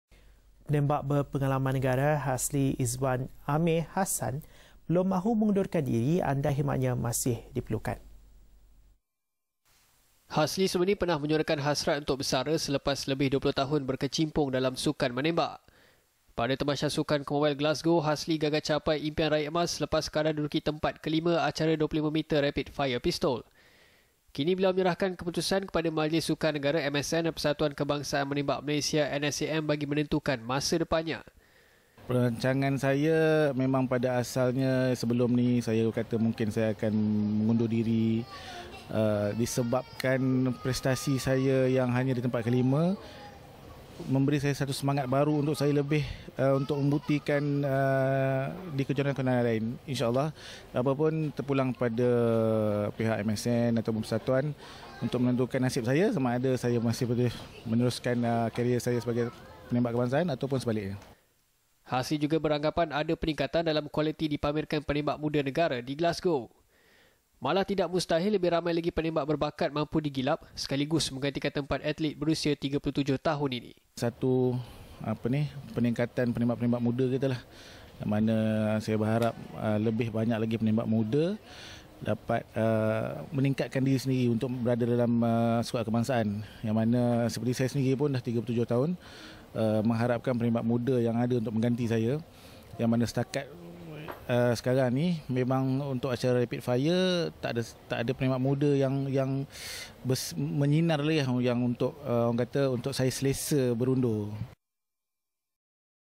Hasli tiada hasrat untuk bersara
Penembak berpengalaman negara, Hasli Izwan Amir Hassan belum mahu mengundurkan diri andai khidmatnya masih diperlukan.